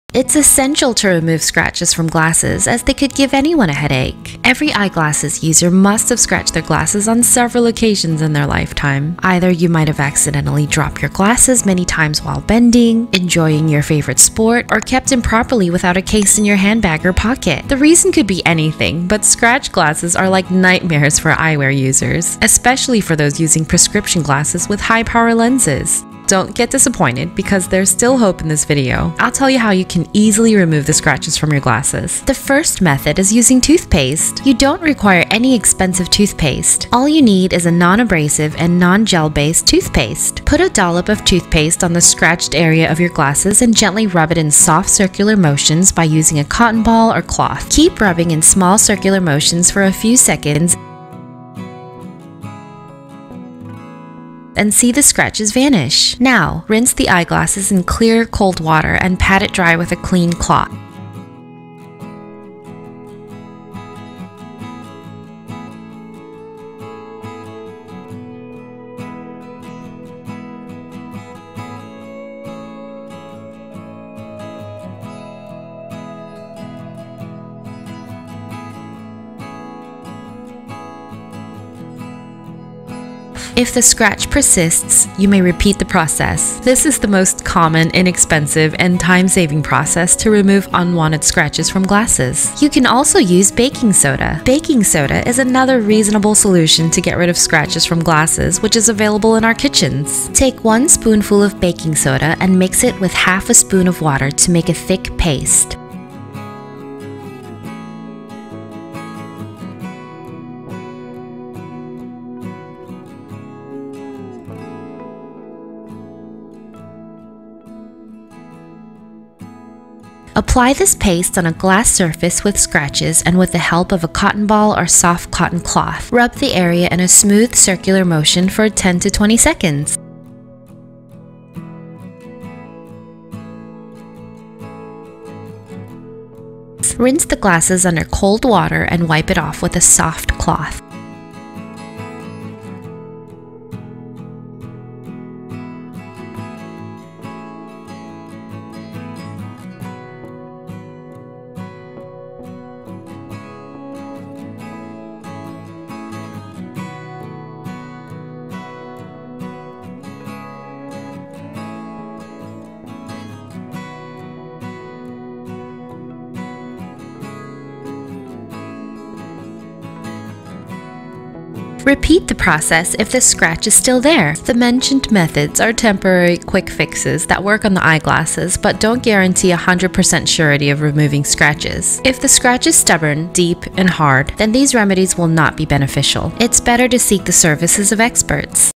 Remove Scratches From Eyeglasses, Sunglasses – Fix And Buff Out Scratches
This home remedy will easily help you guys to get rid of scratches on glasses. So, I hope that you will found this video helpful If yes then make sure to give this video a big thumbs up.